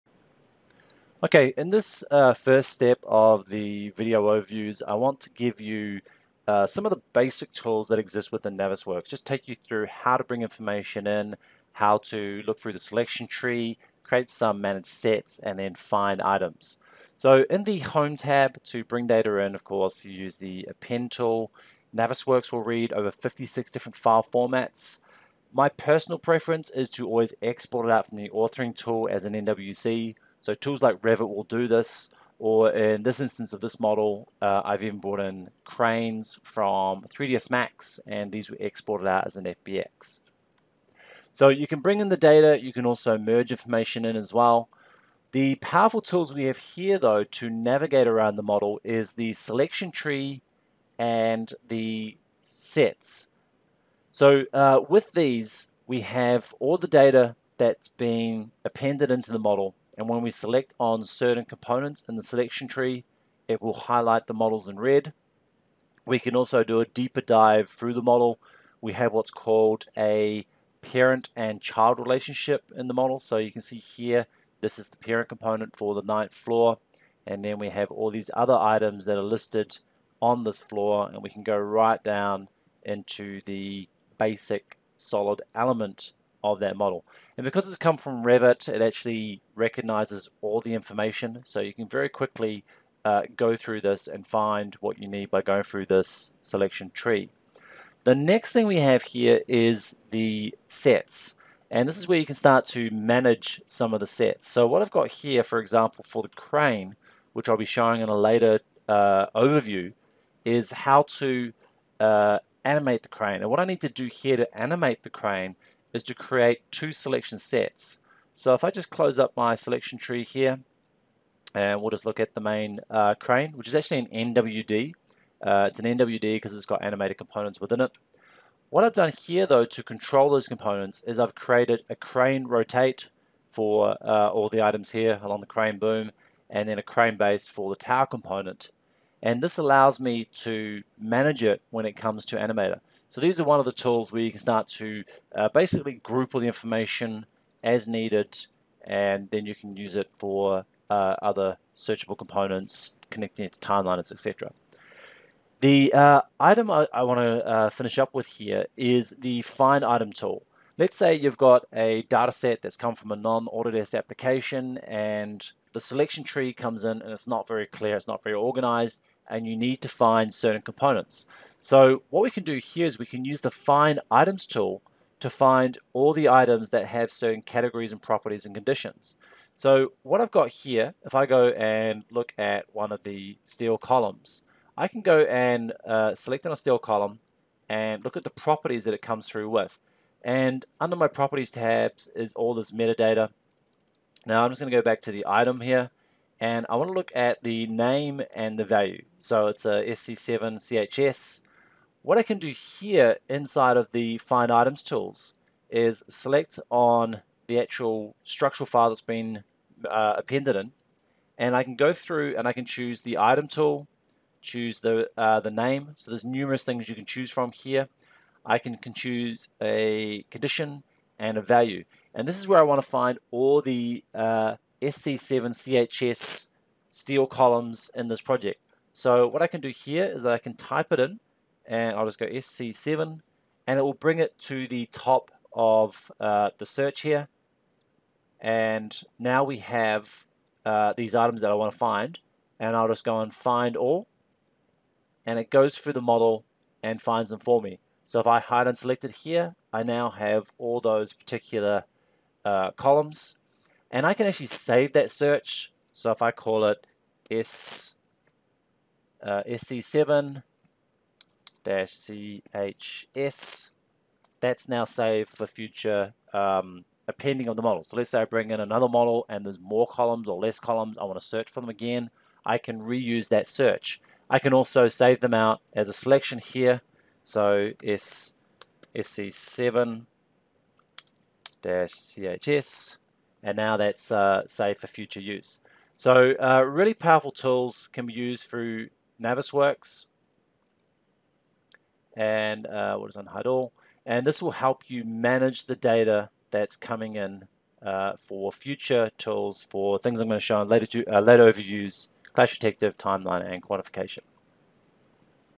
Navisworks Manage 2017 overview of Selection tree, search sets and find items
This video overview will take you through the basics of Navisworks Manages selection tree, sets and find items